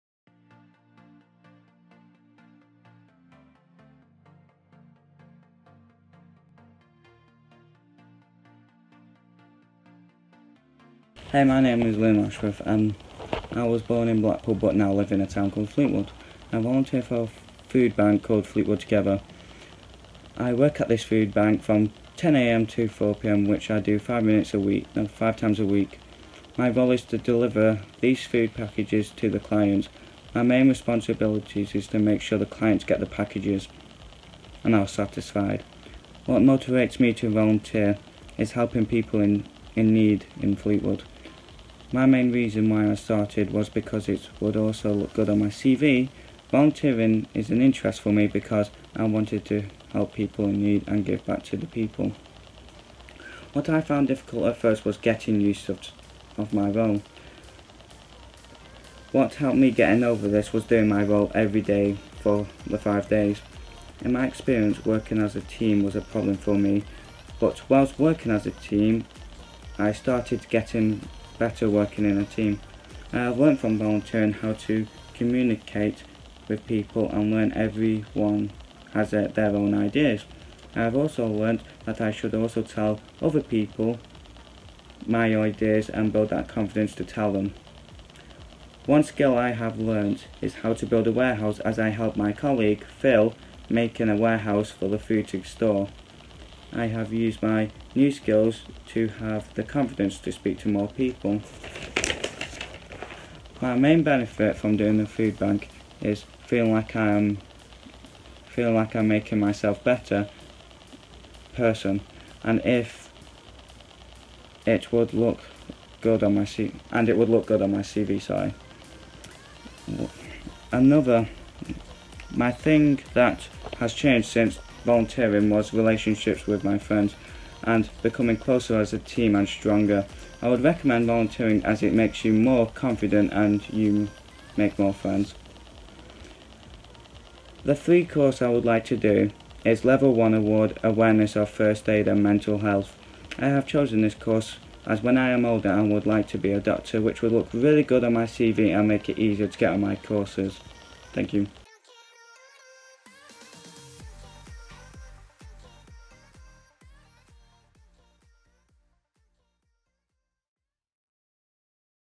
National Volunteers Week- Liam Ashworth from Fleetwood Together Food Bank
As part of National Volunteers Week we recognised and thanked 42 volunteers who have been working hard across Lancashire communities to support others. Because of the dedication and selflessness to help other people around them, we thought it would be nice to thank them. Some of our the volunteers have sent it videos detailing their personal experiences with volunteering.